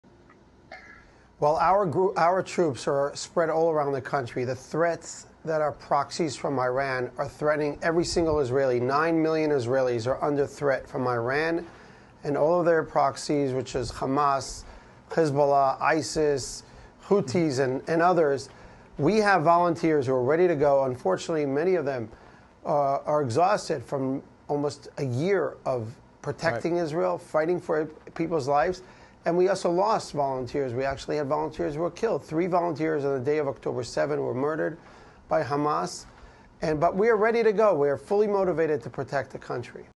Eli Beer on Fox News Discussing Northern War with Hezbollah
Watch Eli Beer on Fox News as he discusses how United Hatzalah volunteers are spread across the country, fully prepared to help and protect Israeli citizens amidst rising tensions. With Operation Northern Arrow in motion and Hezbollah’s missile threat putting millions at risk, our volunteers are standing by, motivated to respond to any emergency during these critical times.